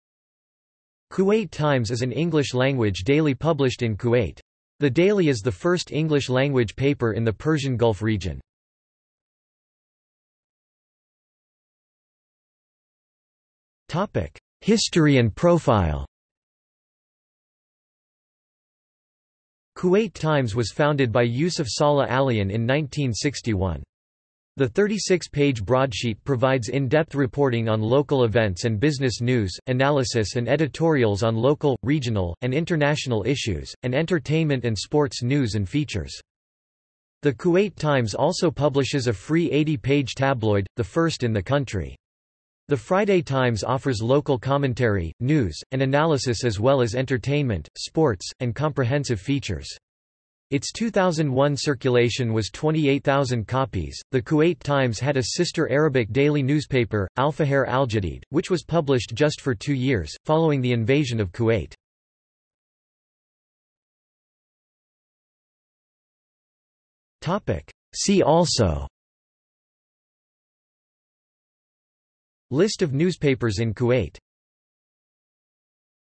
Kuwait Times | Wikipedia audio article
This is an audio version of the Wikipedia Article:
Kuwait Times

00:00:10 undefined
00:01:03 undefined

- Socrates

SUMMARY
Kuwait Times is an English language daily published in Kuwait. The daily is the first English language paper in the Persian Gulf region.